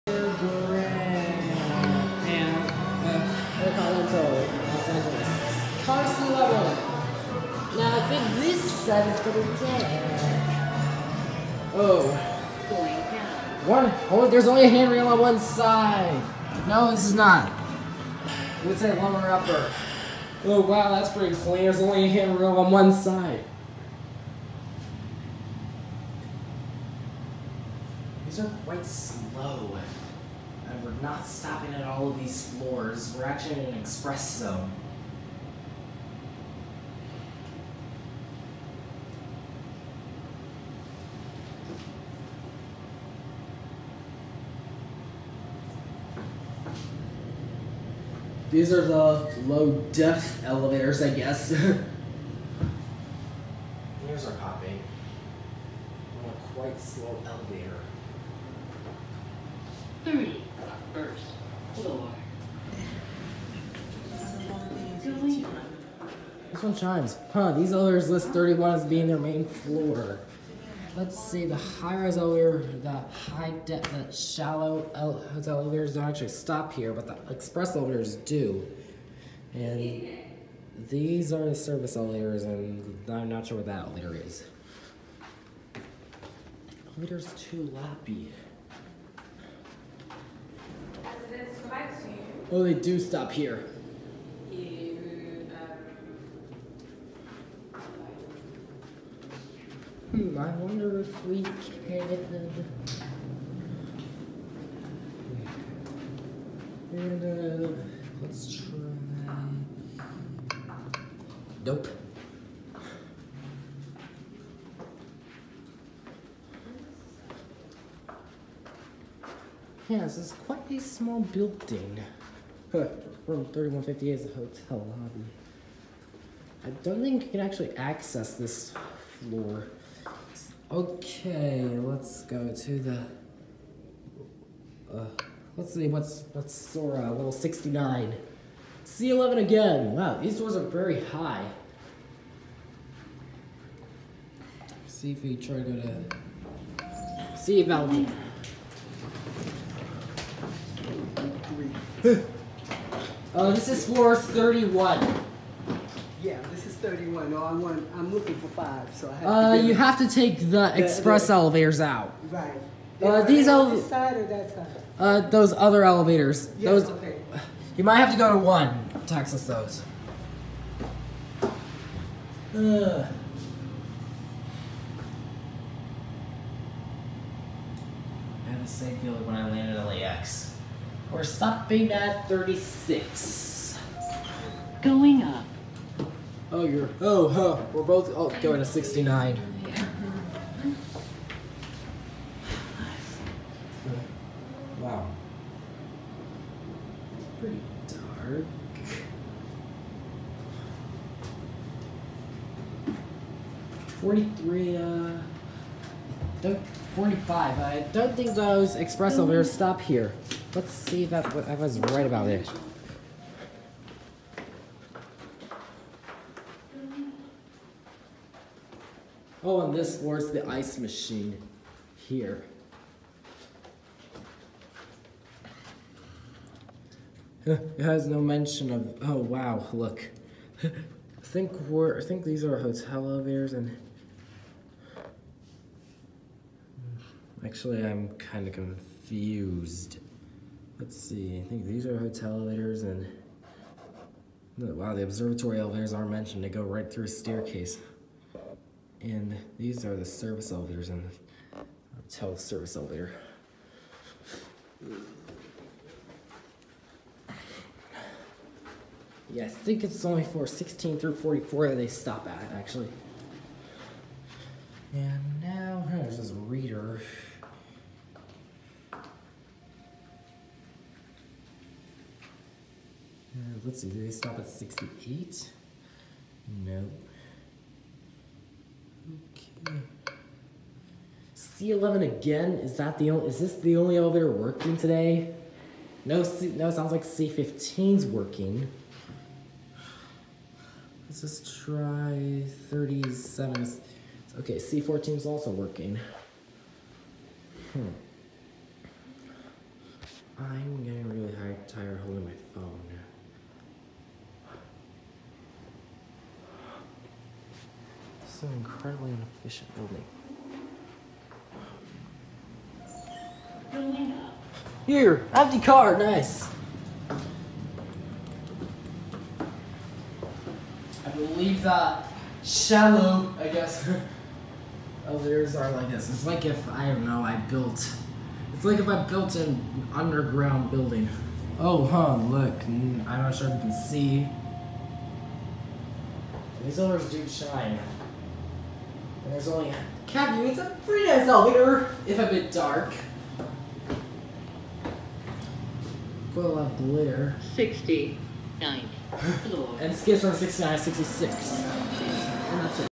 300th!Slow Otis CompassPlus “High-depth” Elevators @ Intercontinental Wilshire Grand Los Angeles, CA
Filmed October 2018. This is a very unique concept but it would be more convenient if you could regularly access the double-deck express elevators from these floors.